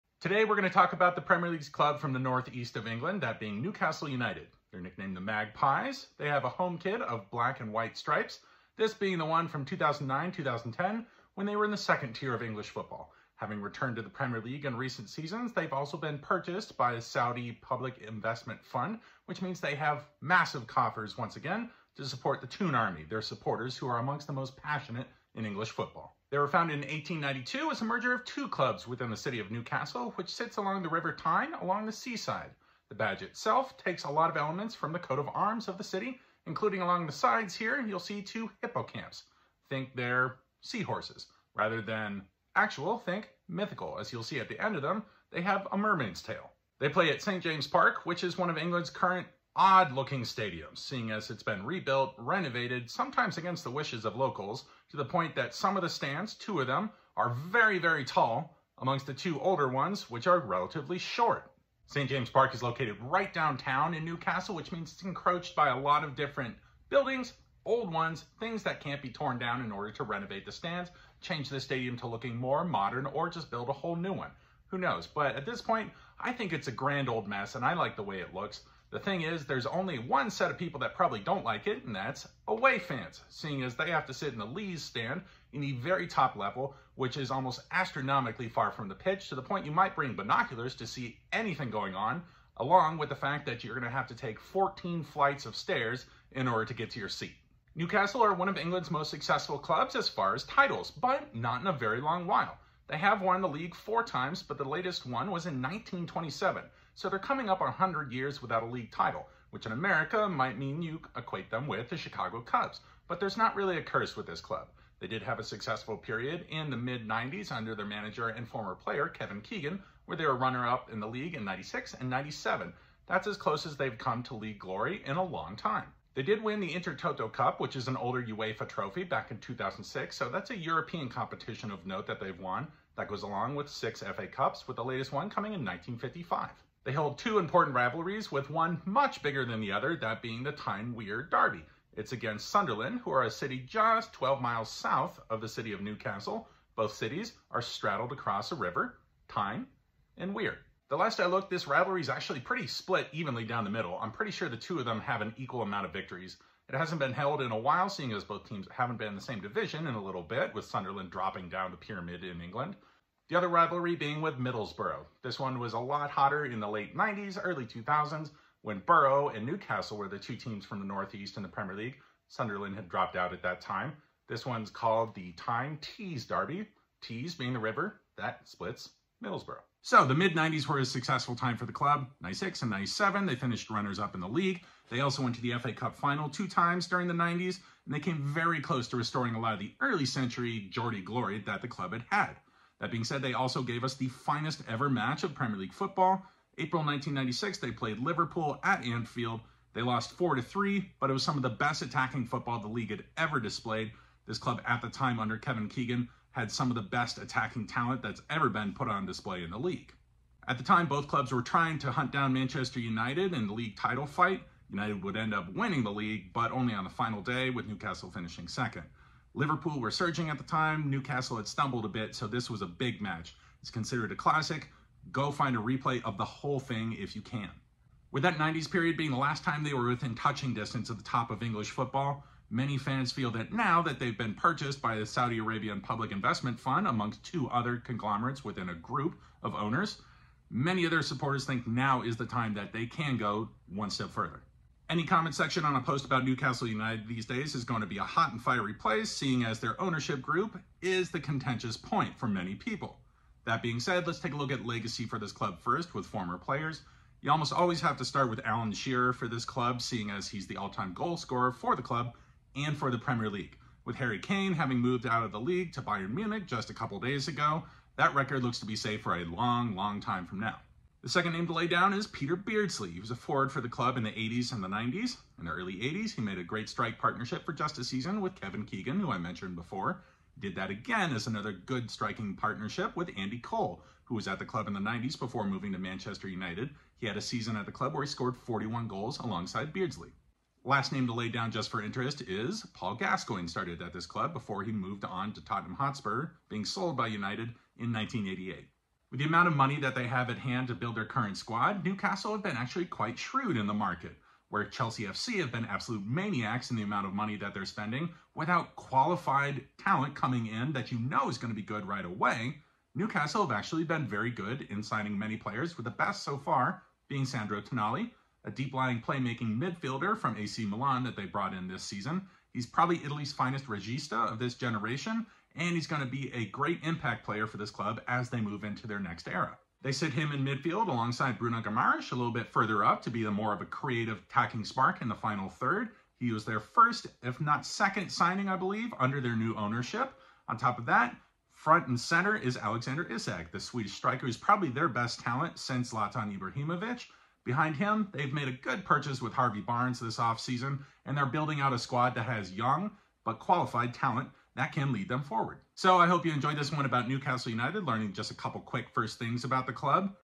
Newcastle United
Club in Focus: Newcastle United. Followed by the passionate Toon Army, this club from the northeast of England is building once again, after their latest period of success in the Nineties ended without a league or FA cup title. newcastleunited newcastle toonarmy england premierleague keegan tonali football soccer futbol @NUFC @premierleague